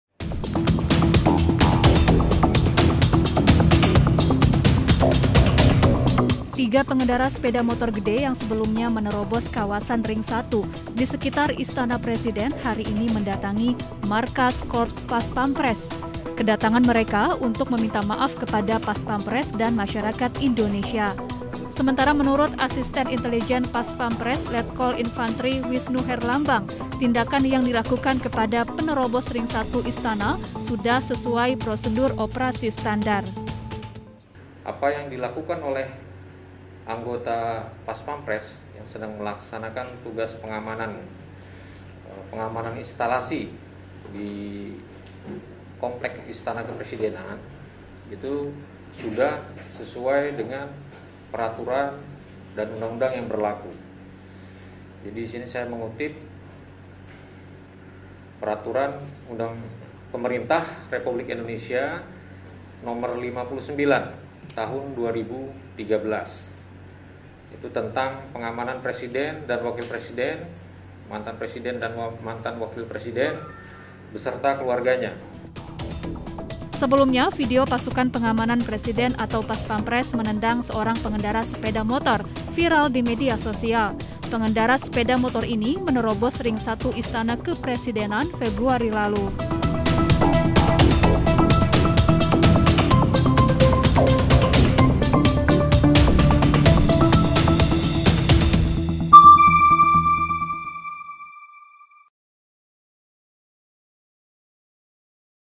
Pengendara Moge Terobos Ring 1 Istana Presiden Minta Maaf
Tiga pengendara sepeda motor gede (moge) yang menerobos kawasan ring satu di sekitar Istana Presiden, Jakarta Pusat, mendatangi Markas Korps Paspampres, Senin (1/3/2021).

Kedatangan mereka untuk meminta maaf kepada Paspampres dan masyarakat Indonesia.

Android: inews.id/android
iPhone: inews.id/ios